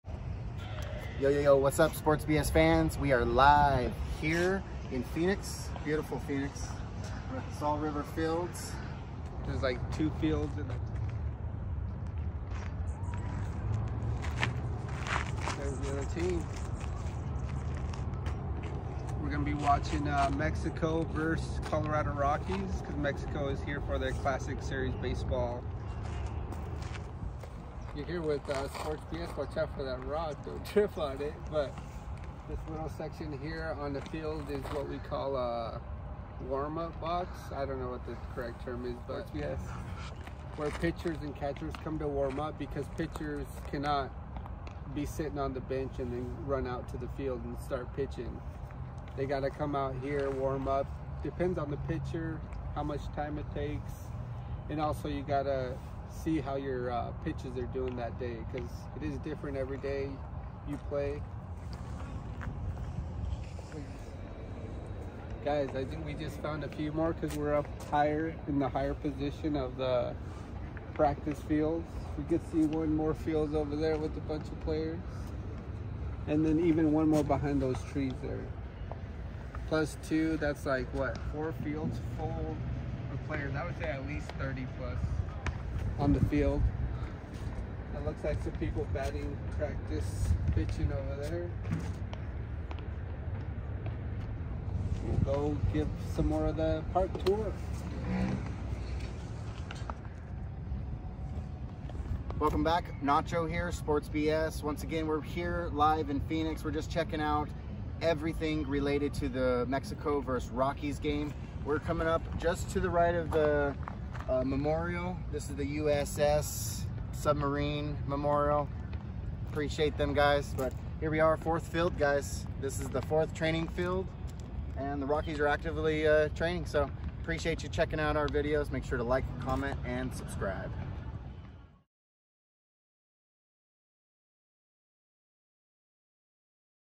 Colorado Rockies spring training facilities
Sports PS takes a day trip to see the Colorado Rockies spring training facility that they share with the Arizona Diamondbacks￼